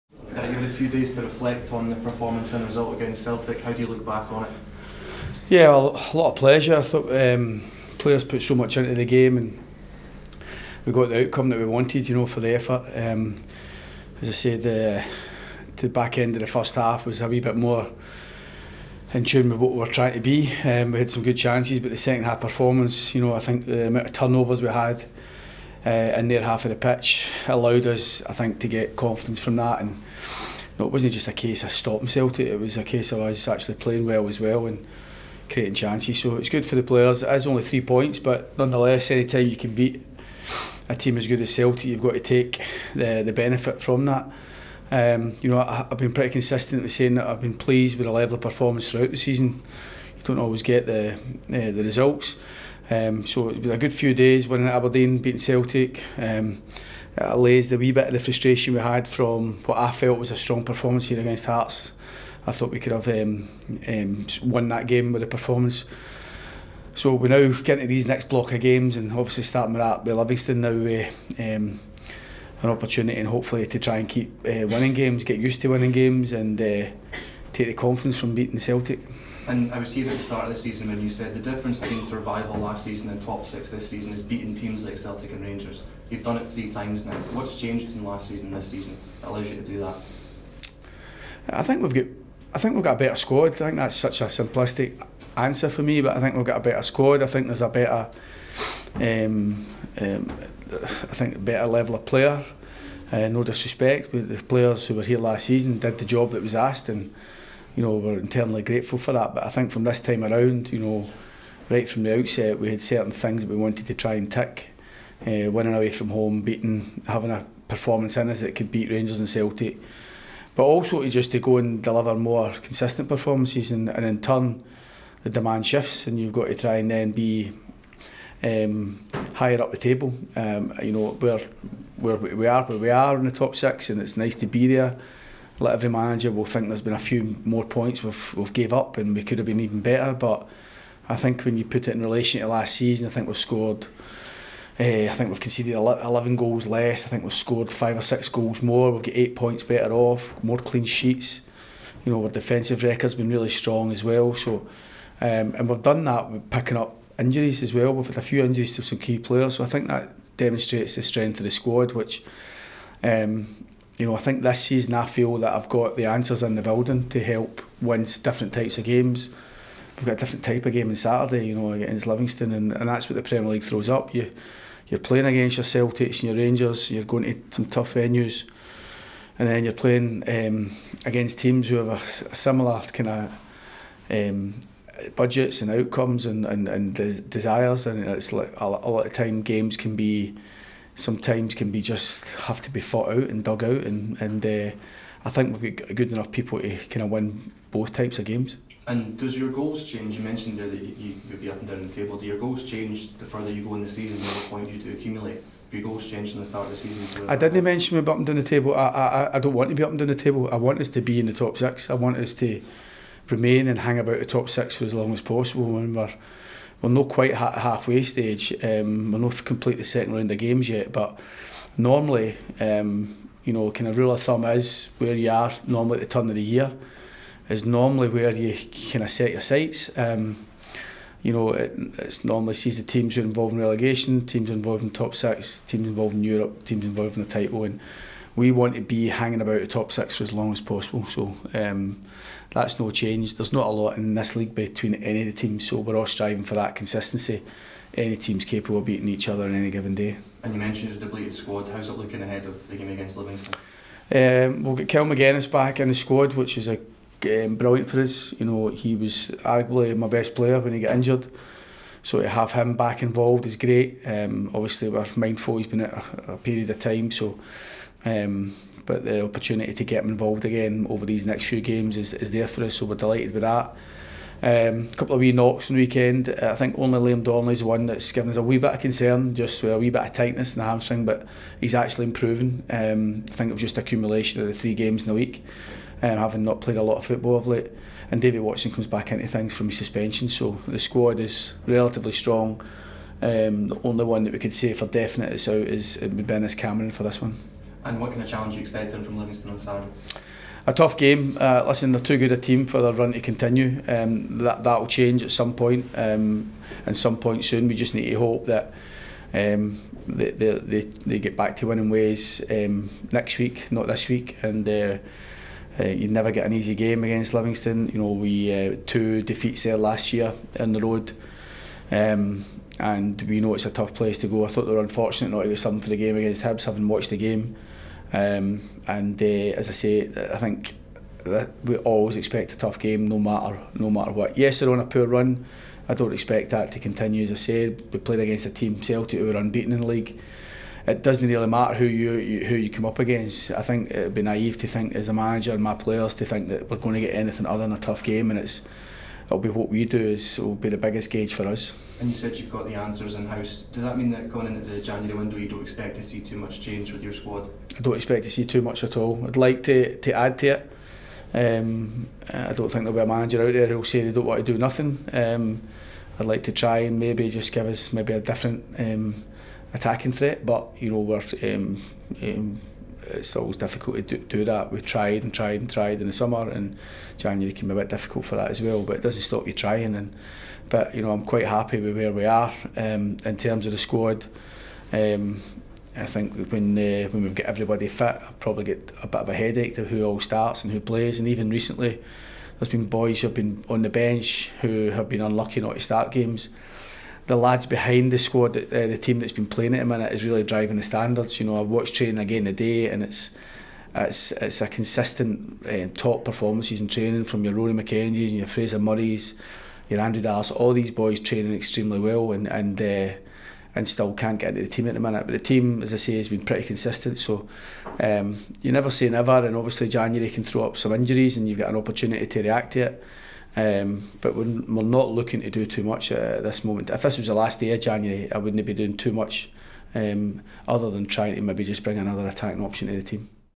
🎙️ McInnes: 'We have enough in the building to win all types of games'
Derek McInnes previews our clash against Livingston on Saturday 16th December.